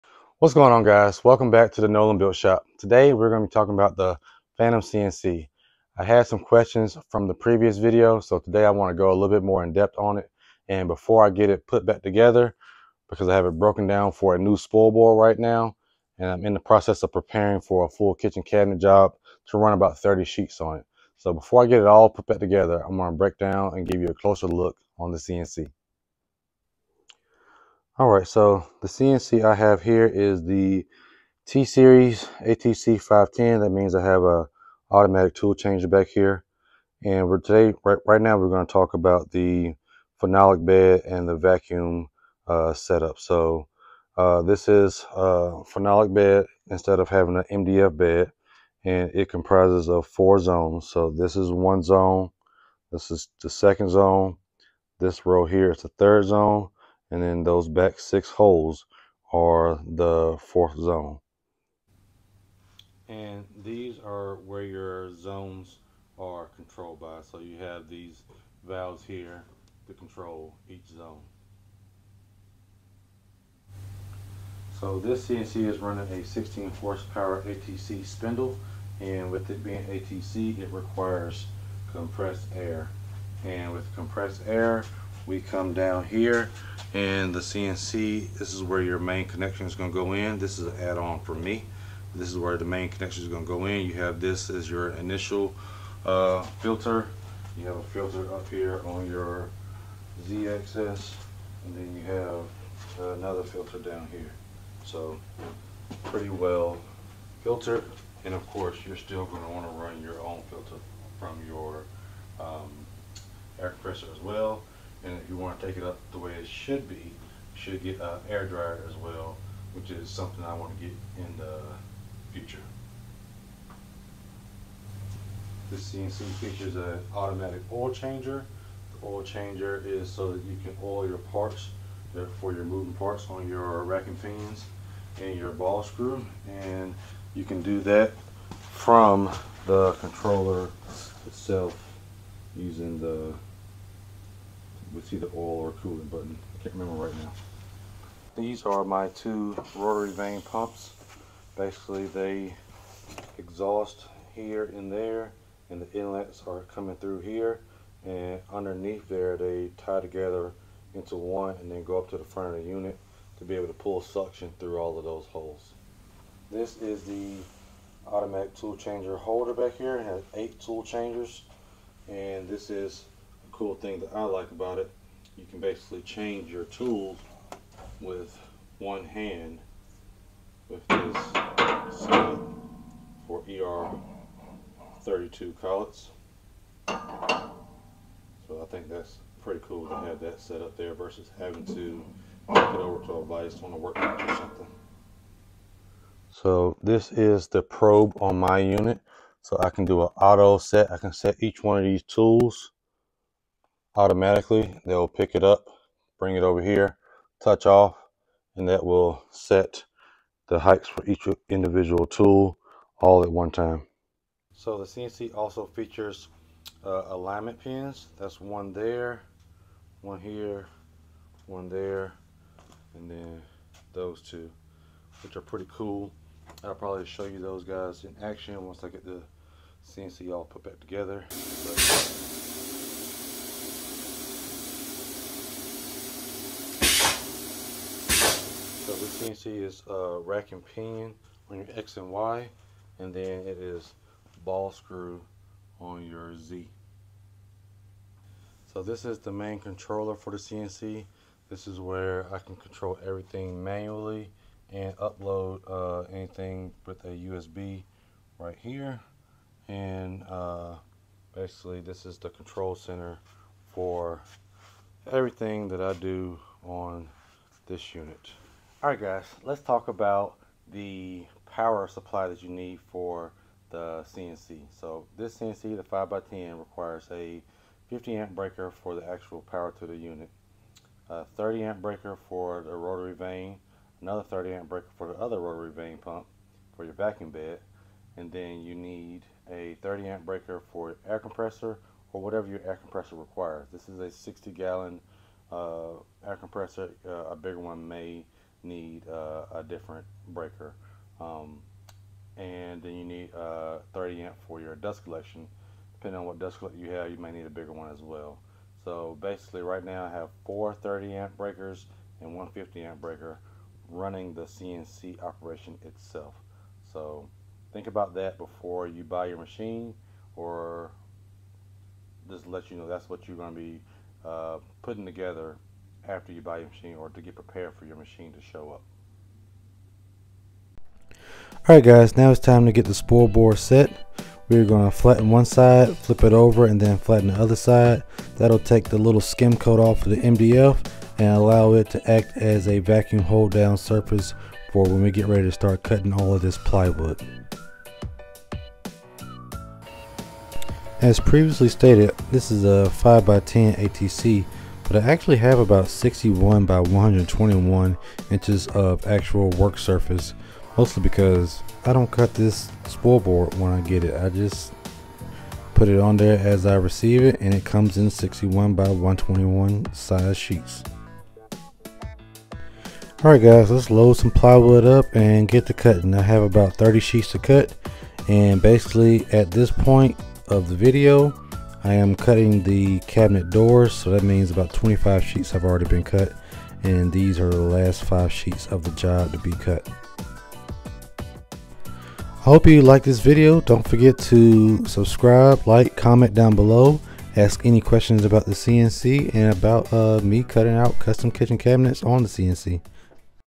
Phantom CNC
I am going to cover some questions from the previous video on the @phantomcncsystems 5 x 10 ATC CNC. This video will go over many of the features of the CNC and some of the requirements like how much power you’ll need to run it.
- timestamps-
00:00 - Start
00:40 - Phenolic vacuum bed
01:16 -  ATC Spindle
02:30 - Vacuum pumps
02:50 - Automatic Tool Changer
03:39 - Alignment Pins
04:30 - Power Supply Requirements
05:34 - Spoilboard surfacing